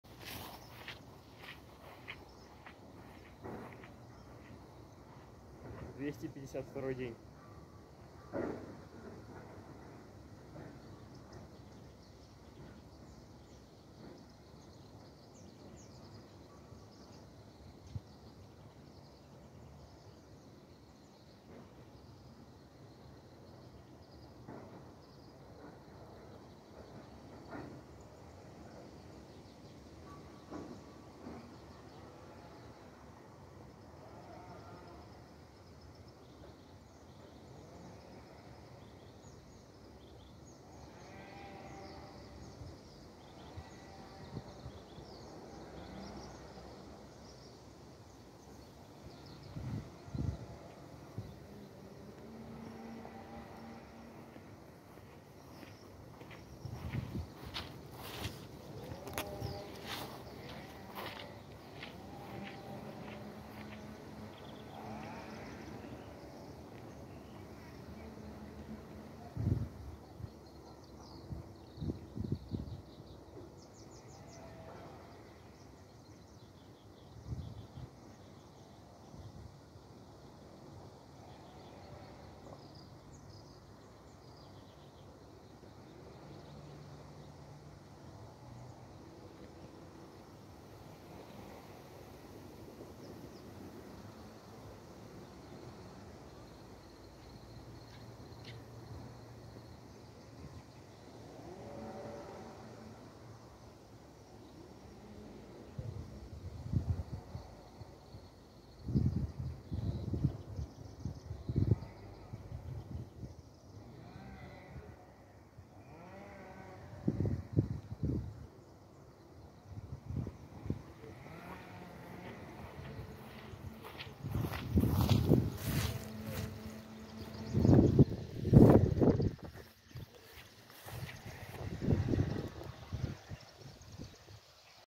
Подтягивания каждый день | День 252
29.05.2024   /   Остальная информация
План тренировки:
Подтягивания различными хватами:  5х10/12
Отжимания на брусьях различной ритмики: 5х15/20
Приседания с собственным весом:  5х20
Отдых между повторениями  30/60 секунд